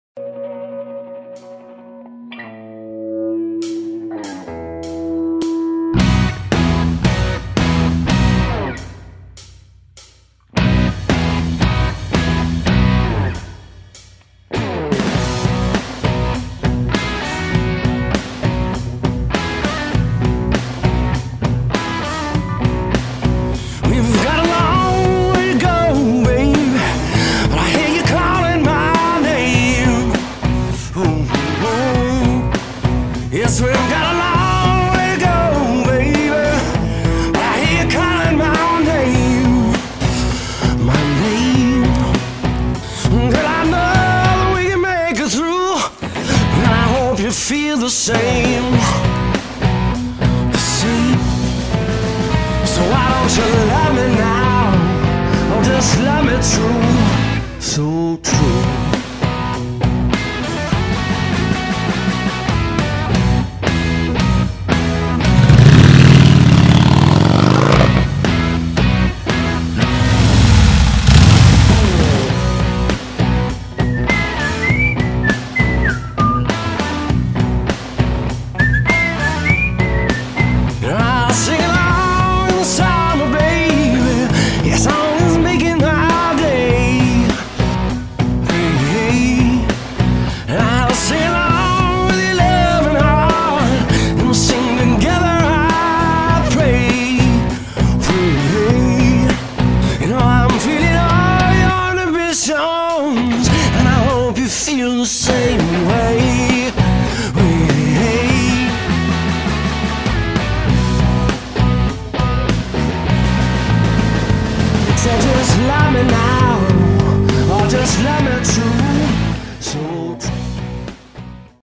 K&N Softail by RSD Video
Filtration is an often overlooked part of your bikes life and performance. K&N takes that job very serious, so much so they had us build a bike to show off K&N performance filtration products. This machine has a solid blend of performance and style for a machine that truly reflects the K&N attitude.

Shot and edited by Joseph Hitzelberger